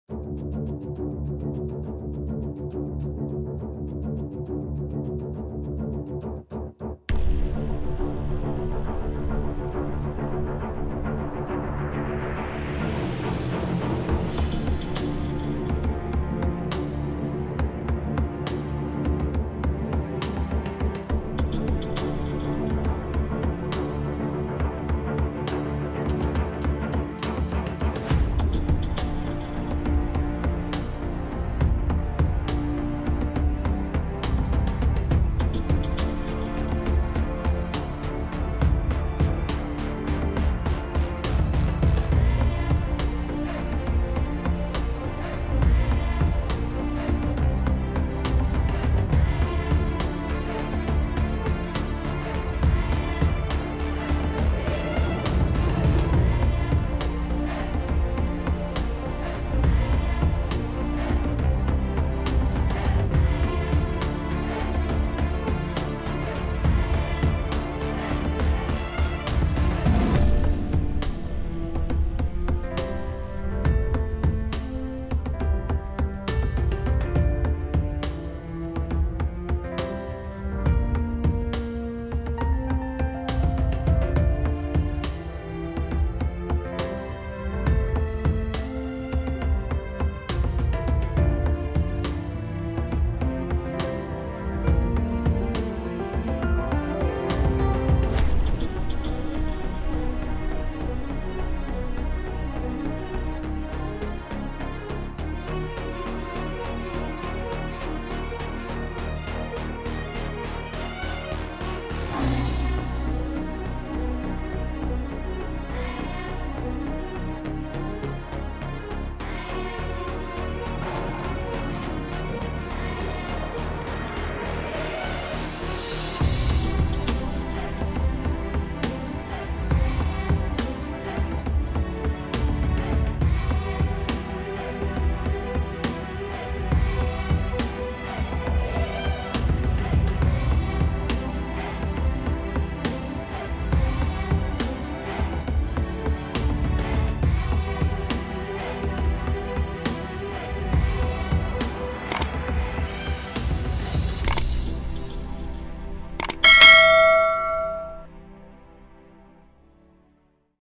Perundingan Buntu, Israel Sebut Tuntutan Hamas Khayalan
Media Israel menyebut pembicaraan tak langsung antara Israel dan Hamas di Qatar menemui jalan buntu. Channel 12 Israel melaporkan, pemerintah telah menarik tim perunding mereka di ibu kota Qatar, Doha.

Kantor Perdana Menteri Benjamin Netanyahu menyatakan pihaknya tidak akan menuruti tuntutan khayalan Hamas.

Israel juga menuduh Hamas tidak tertarik untuk melanjutkan perundingan usai diduga menolak komproni yang ditengahi Amerika Serikat.

Penulis Naskah: Novyana Nurmita Dewi
Video Editor: Novyana Nurmita Dewi
Produser: Farid Firdaus